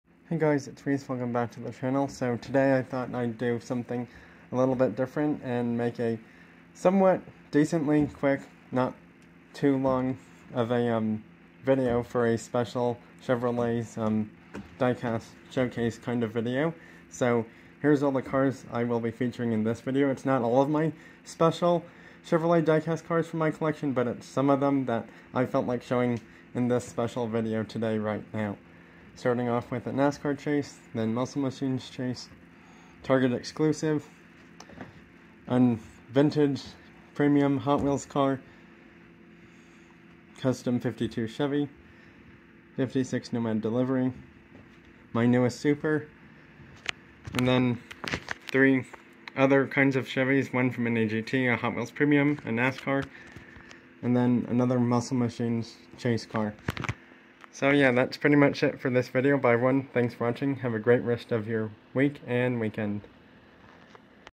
Special Chevy diecast showcase video!!
Showing some special or unique Chevrolet diecast cars today in a showcase video!!